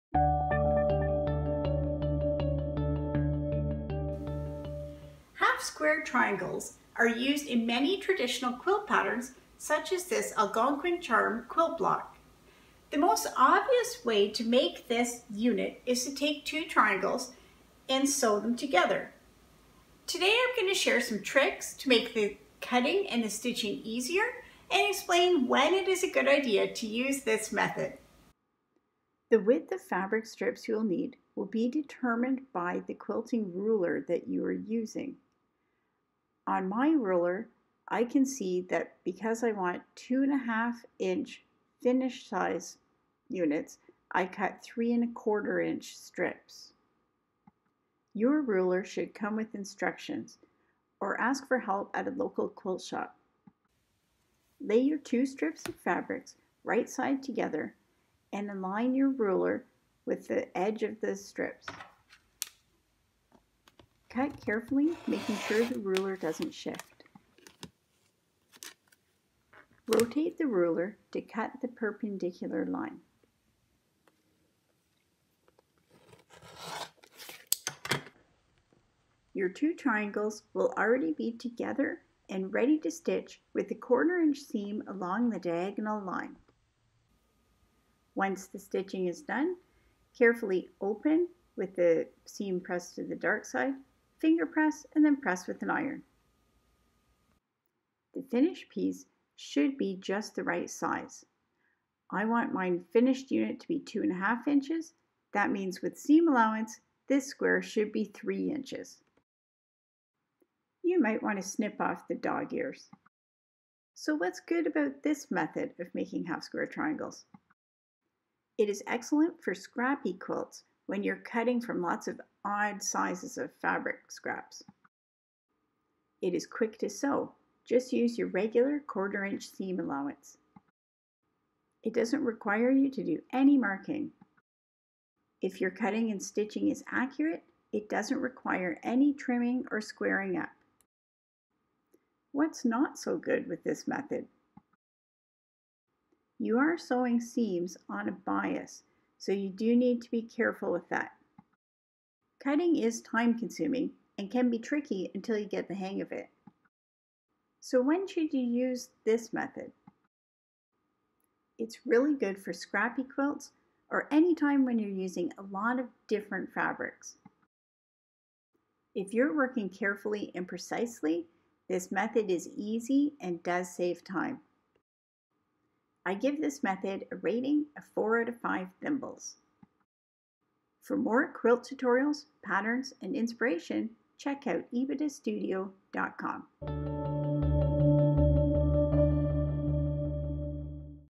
Making half-square triangles with the Obvious method
To new quilters, the most obvious way to make half square triangles together is to sew two triangles together. This isn't always the best method, but sometimes it's just what you should do.

Find out when to use this method and tricks to help it be accurate.

Chapters
0:00 introduction
0:30 fabric requirements
1:00 cut
1:24 stitch
1:34 press
1:44 measure
1:55 what's good about this method?
2:22 what's not good about this method?
2:40 when should you use this method?
2:56 rating